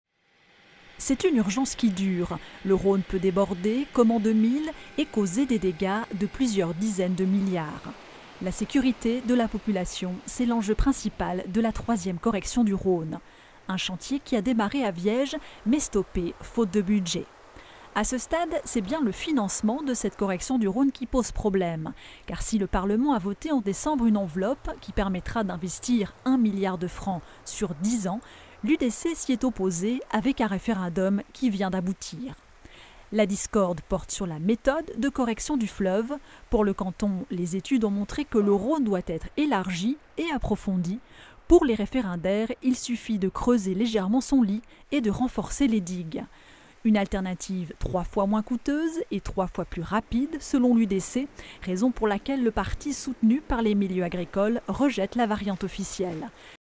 Rhône 3: les enjeux d’un dossier à plusieurs milliards
Le Rhône, en Valais, doit être sécurisé pour la troisième fois depuis le XIXe siècle. Le Gouvernement valaisan a été suivi par tous les partis sur la méthode et le financement de ce chantier pharaonique, à l'exception de l’UDC. Le dernier mot appartiendra au peuple avec un vote dont les enjeux portent sur plusieurs milliards.

Retrouvez notre dossier "RHÔNE 3 - LES VALAISANS VOTERONT" sur notre site internet